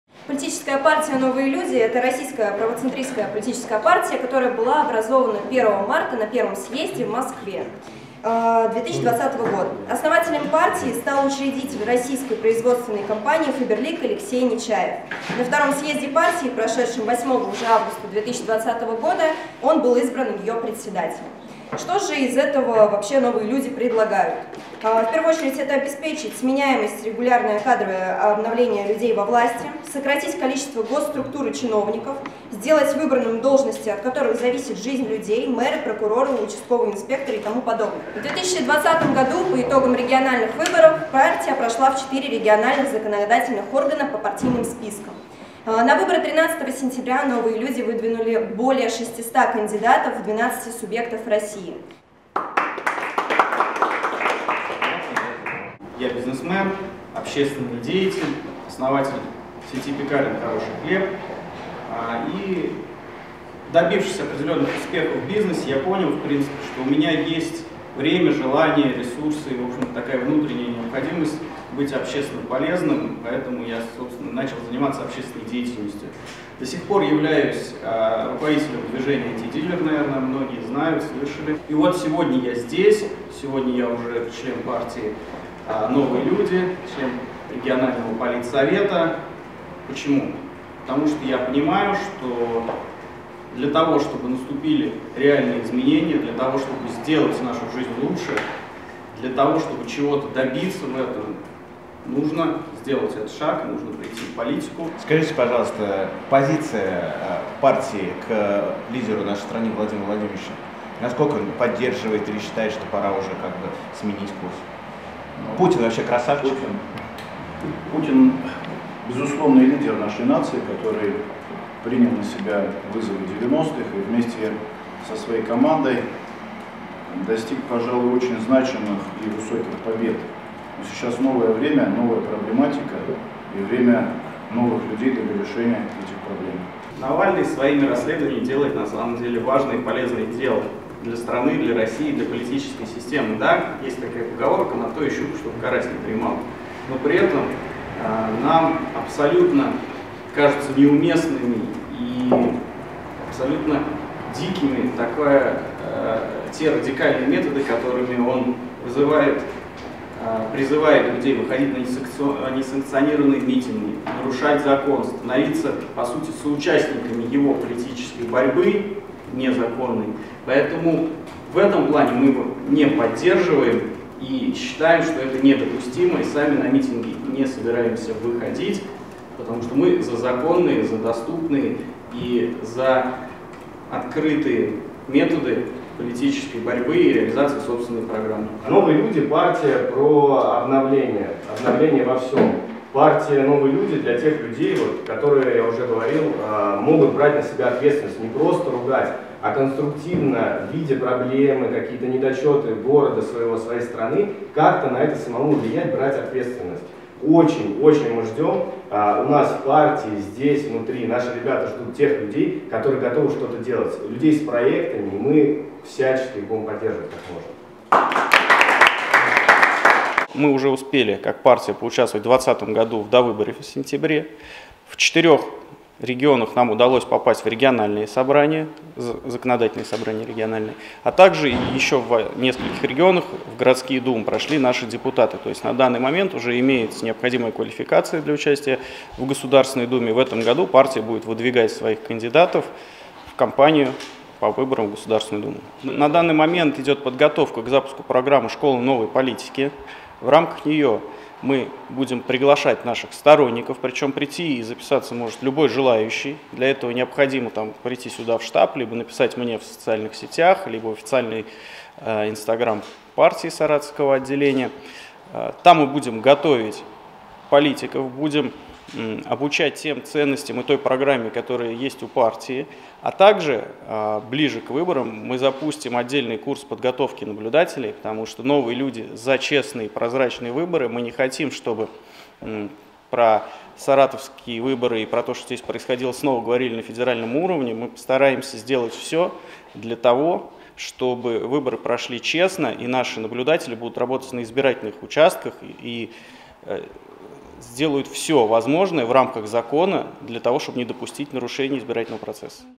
Саратовские "Новые люди" о партии и выборах
"Новые люди" намерены выдвинуть кандидатуры на сентябрьских выборах в Госдуму и Саратовскую городскую думу по одномандатным округам.

Об этом руководители регионального отделения партии заявили сегодня на официальной презентации в областном центре.  Подробности на сайте ИА "Взгляд-инфо".